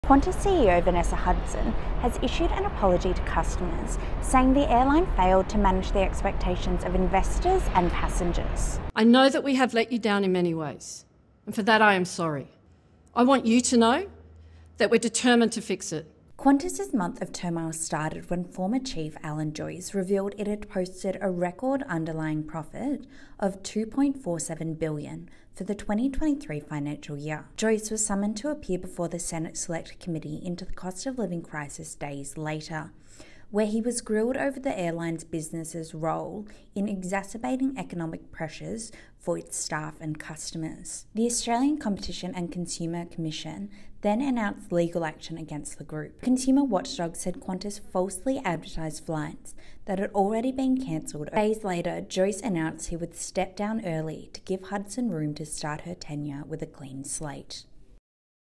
‘We have let you down in many ways’: Qantas CEO says sorry to customers
Qantas boss Vanessa Hudson has committed to a comprehensive review of all “outdated” customer policies after issuing an apology on behalf of the business for failing to strike the balance between managing the expectations of passengers and investors.